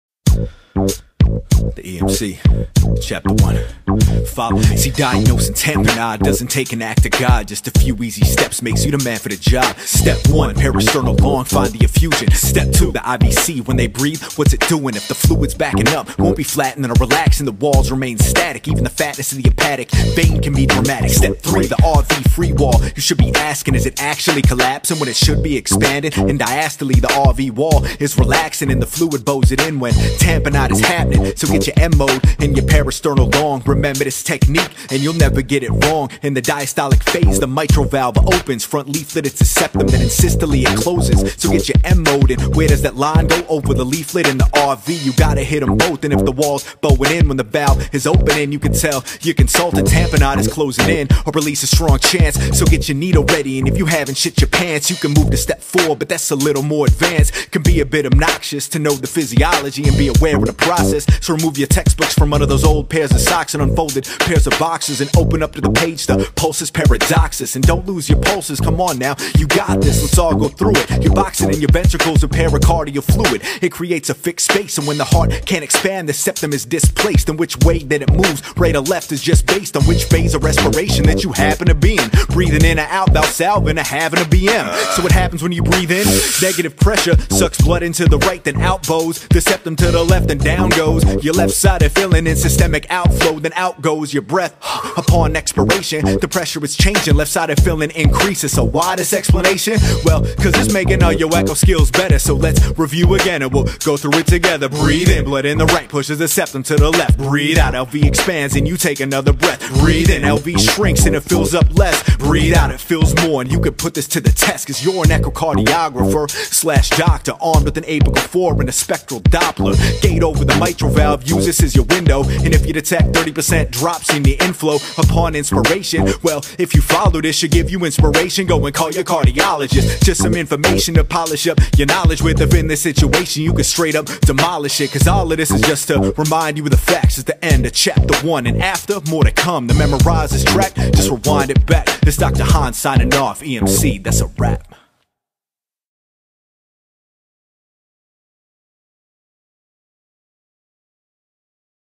The EMC Chap 1 Tamponade
The EMC, where Emergency Medicine (EM) and eMCeein' come together. Chapter 1 - diagnosing cardiac tamponade by ultrasound. Please visit theemc.org for more information.